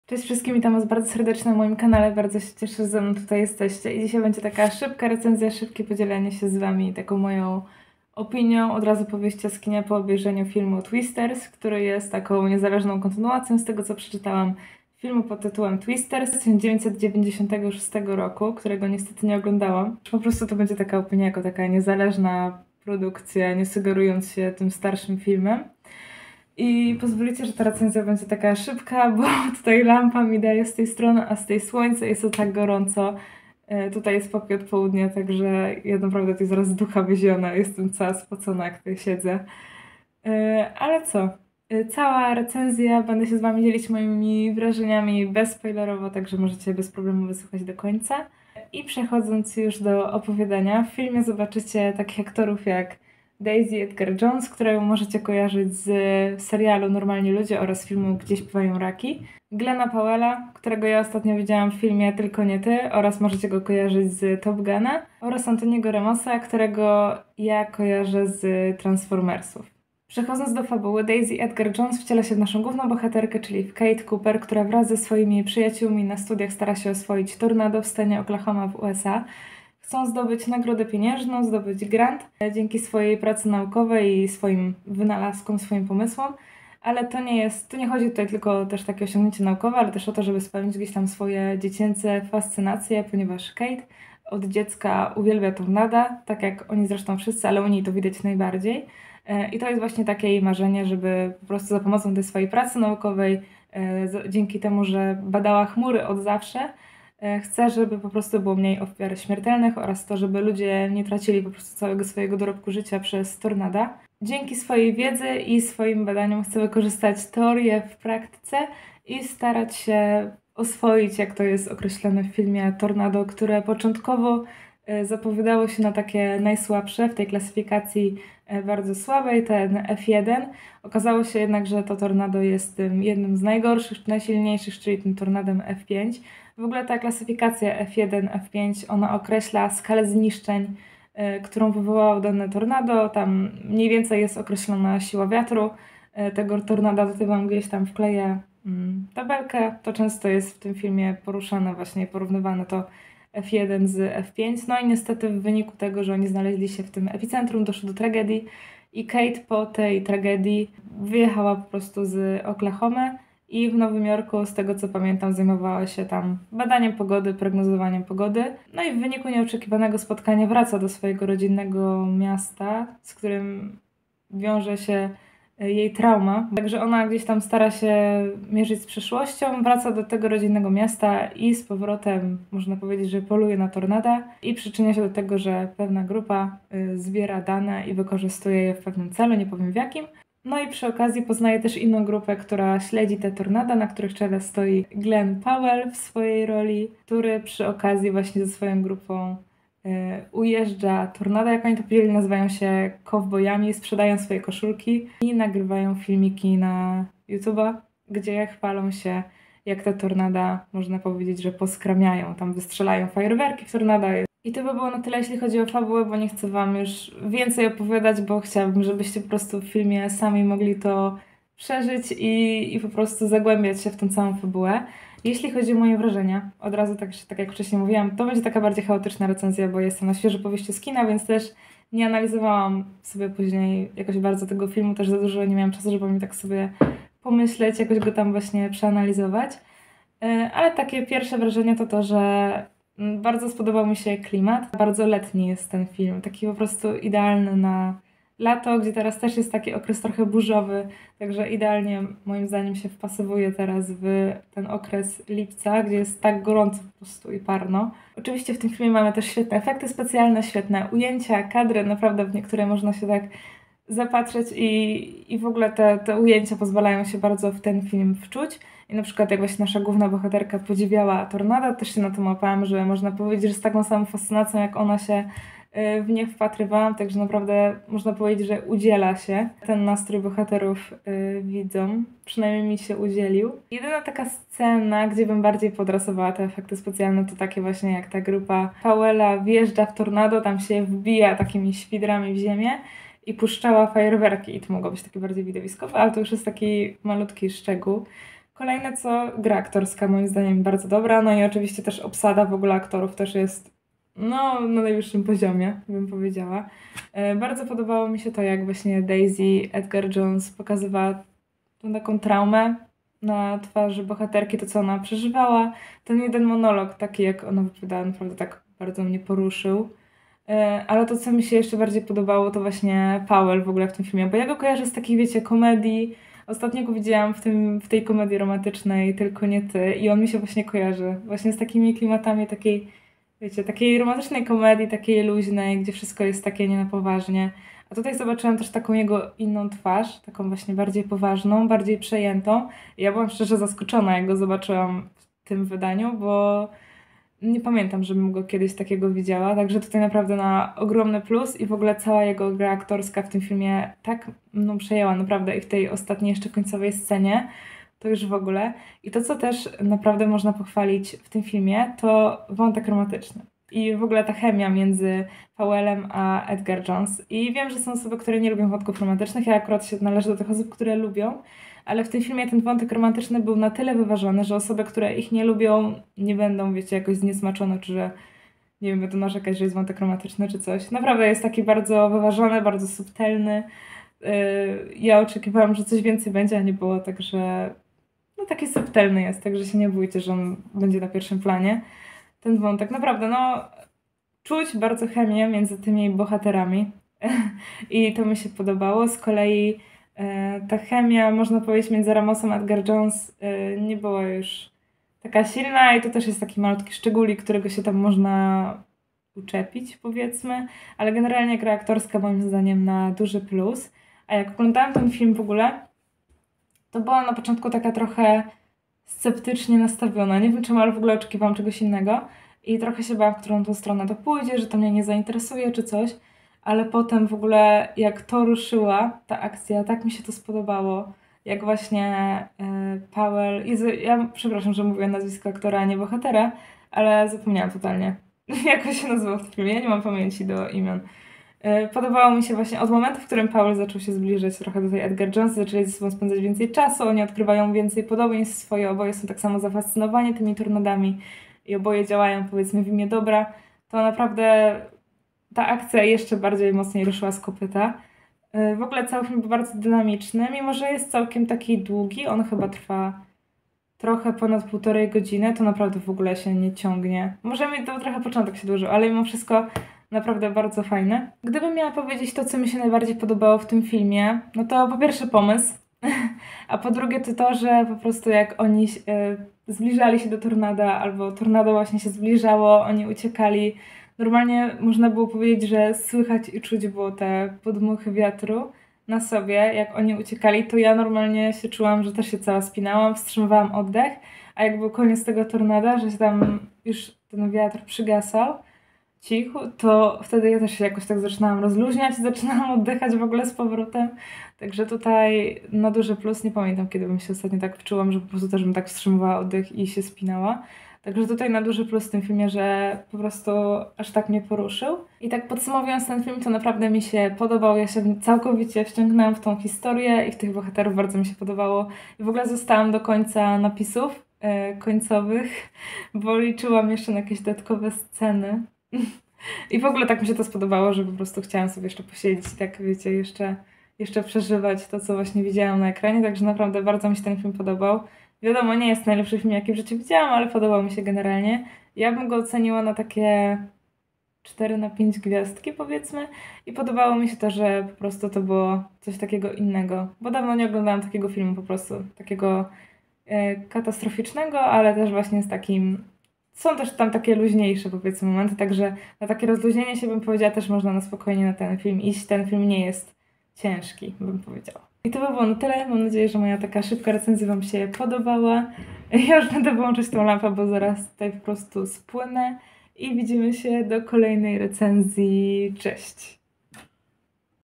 TWISTERS ZAPIERA DECH W PIERSIACH🌪️PIERWSZE WRAŻENIA BEZSPOJLEROWO!
RECENZJA NAJNOWSZEGO FILMU ,,TWISTERS" w reż. Lee Isaac Chung, będącego niezależną kontynuacją filmu ,,Twister" z 1996r. W najnowszej odsłonie grają: Daisy Edgar - Jones (Kate Cooper) oraz Glen Powell ( Tyler Owens) 🌪️ [RECENZJA BEZSPOJLEROWA]
Miłego słuchania 💗
ajlonka